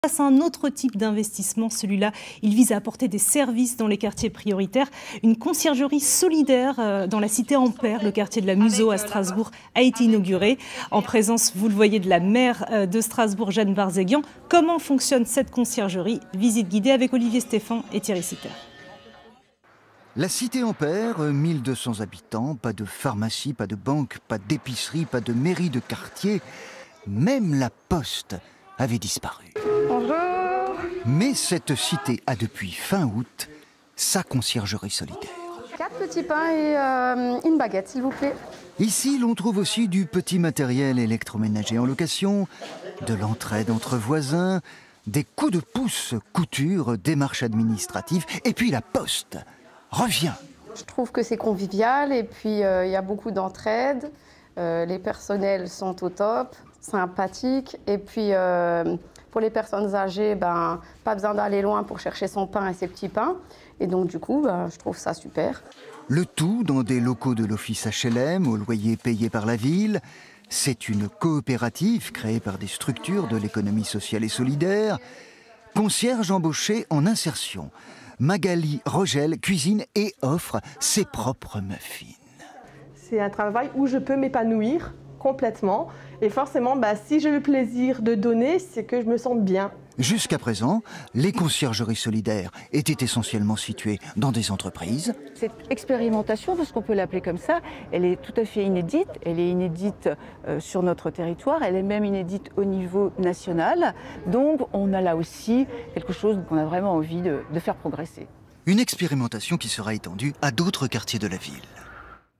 Strasbourg : une conciergerie solidaire au service d'un quartier
Cette idée vise à apporter des services dans les quartiers prioritaires. Une conciergerie solidaire à la cité Ampère, au quartier de la Musau, à Strasbourg, a été inaugurée. Comment fonctionne cette conciergerie ? Les réponses dans notre sujet du jour.